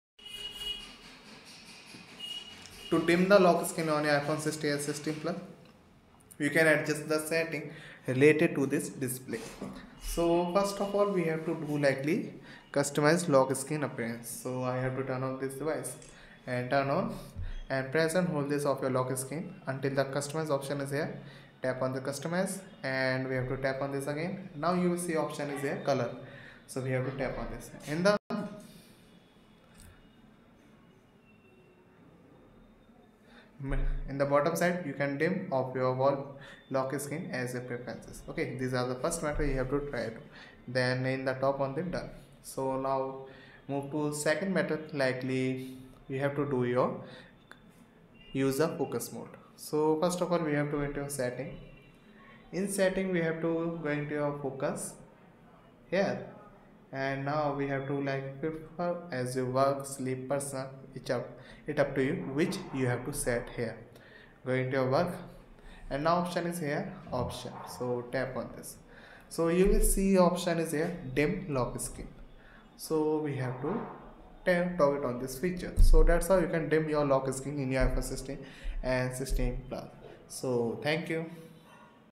iPhone 16/16 Plus: How To Dim Lock Screen
In this informative video, we will guide you through the process of dimming the lock screen on your iPhone 16 and iPhone 16 Plus. Discover the simple steps to adjust your device's display settings for a more comfortable viewing experience.